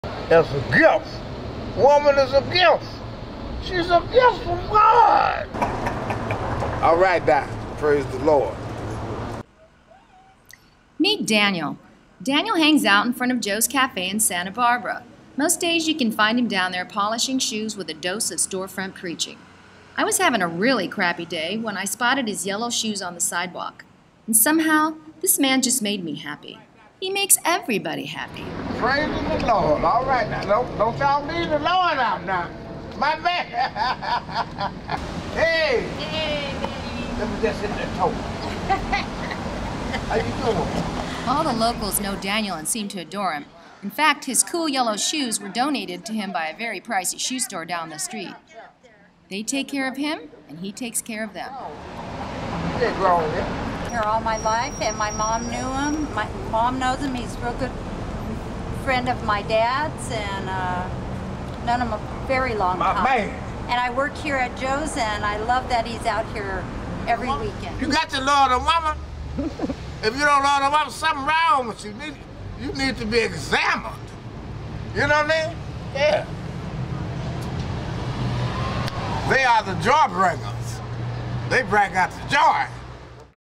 Santa Barbara- The Joy Bringer
A well loved Santa Barbara, Ca local icon, Daniel has been offering shoeshines with a dose of storefront preaching here for 37 years. He is a delight! This was my second student film for TCA at Brooks Institute.